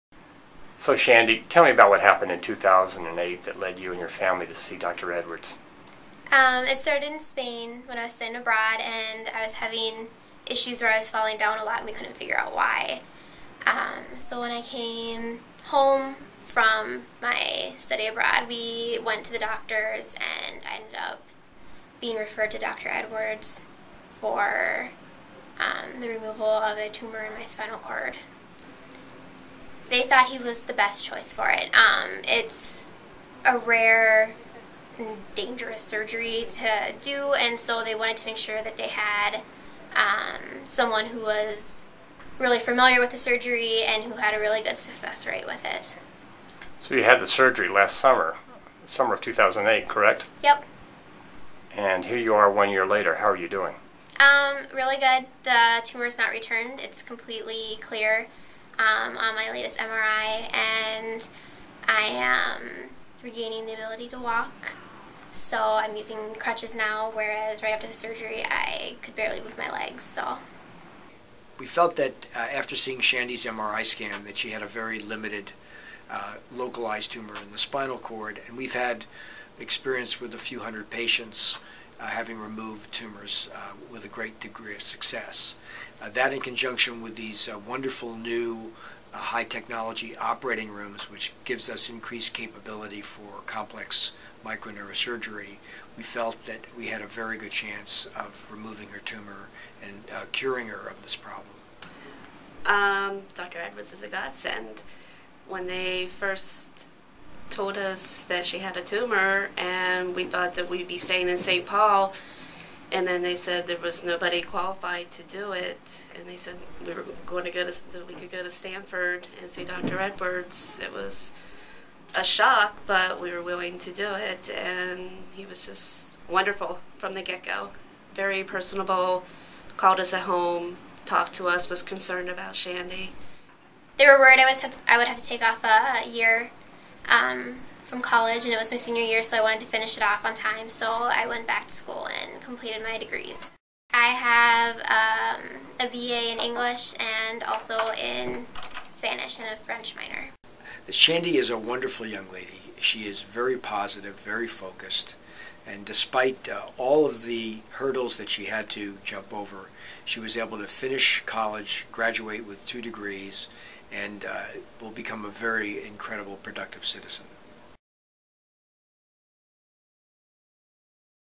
Shandi Wagner & Dr. Michael Edwards, spinal cord tumor surgery, Lucile Packard Children's Hospital
Shandi Wagner, 22, is a double-major graduate of the University of St. Thomas in St. Paul, MN, Class of 2009. This is a remarkable achievement, especially since Shandi underwent surgery to remove a spinal cord tumor in 2008. Doctors in Minnesota referred Shandi to the pediatric neurosurgery program of Michael Edwards, MD, chief of pediatric neurosurgery at Lucile Packard Children's Hospital at Stanford. In this June 18, 2009 video, Shandi, her mom Carol and Dr. Edwards discuss the surgery and recovery.